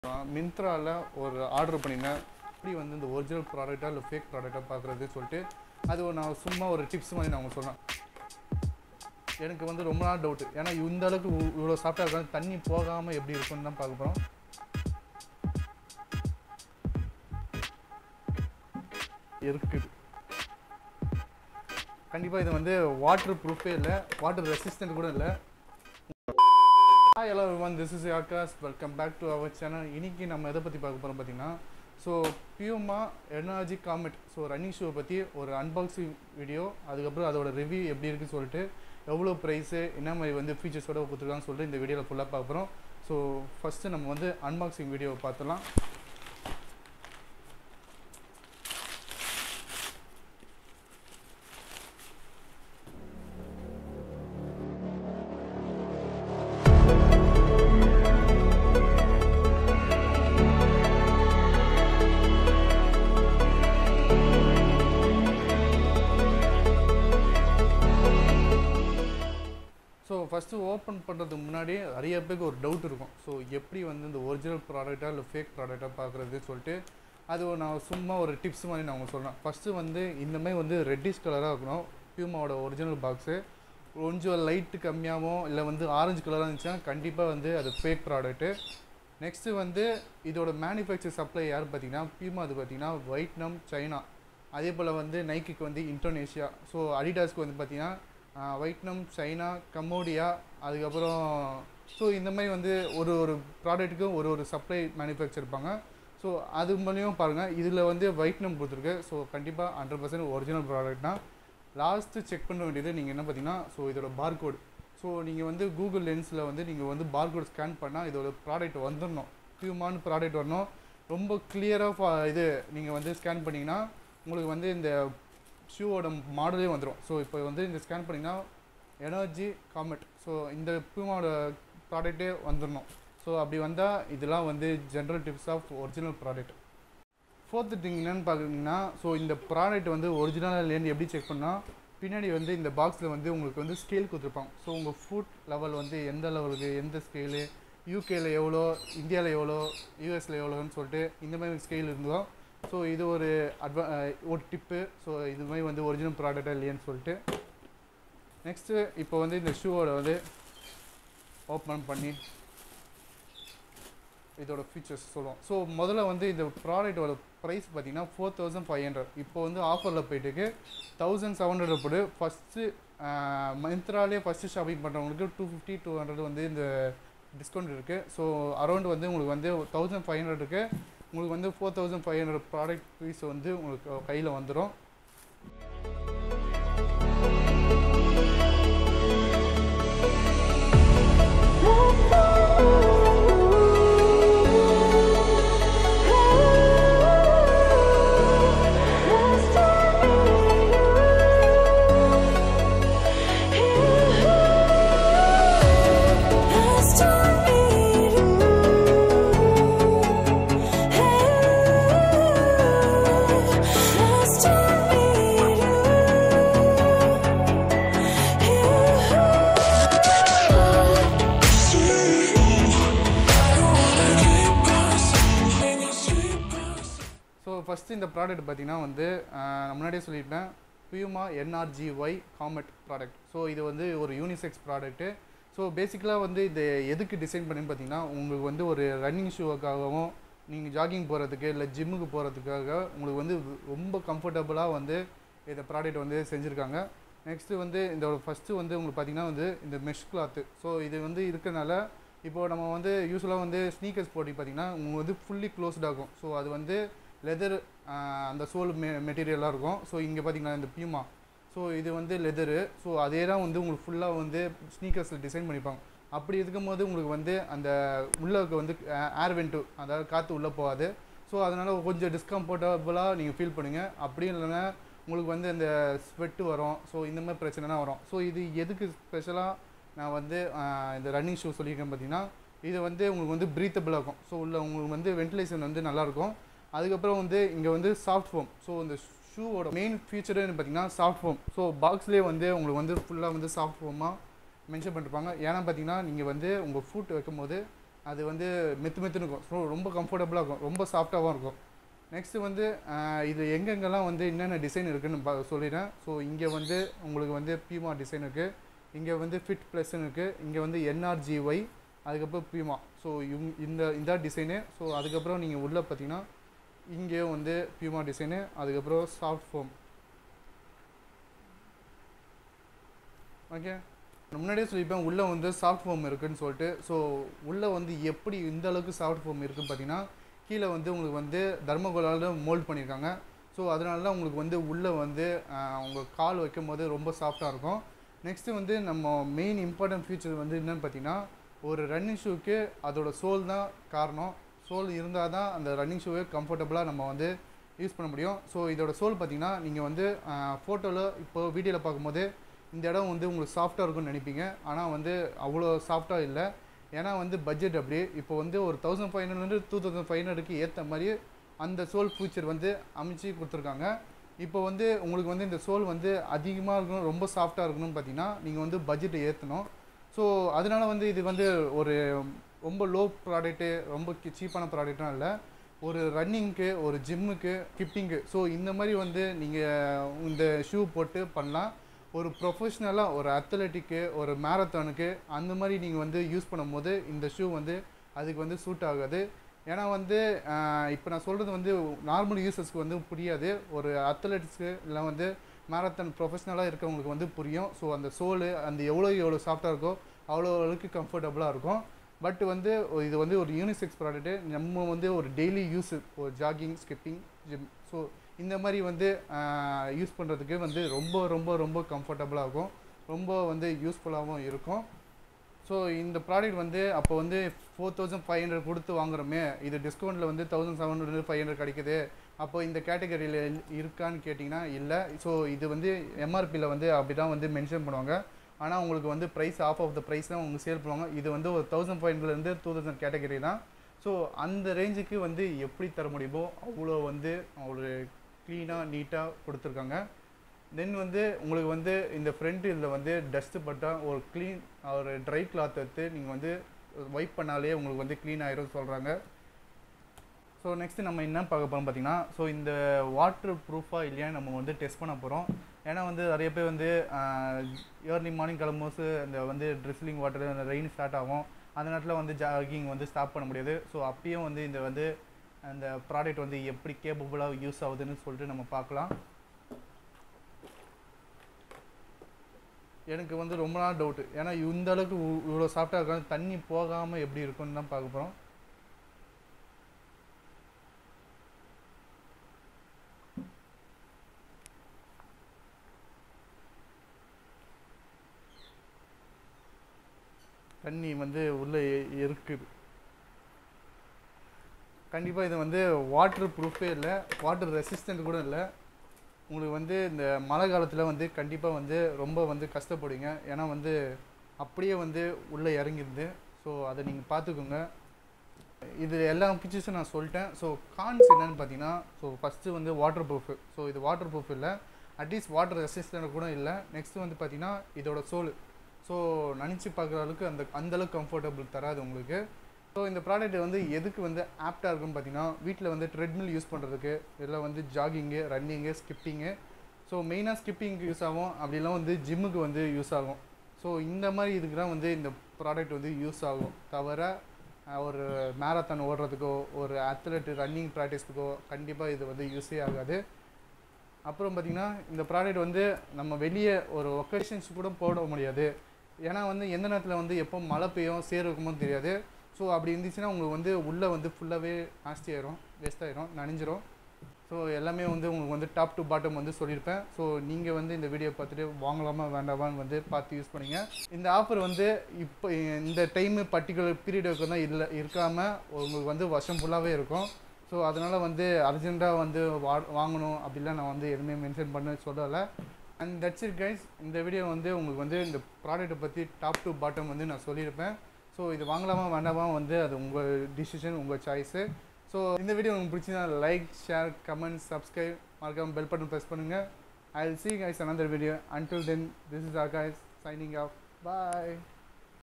Puma NRGY Comet | Myntra |Running Shoe | Soft Foam
In this video I have reviewed  this puma Nrgy comet running shoe worth 2500, watch the whole video for the pros and cons

Puma NrgyComet 👇

Puma Boy's Sneakers

Puma Men's Nrgy Comet Running Shoes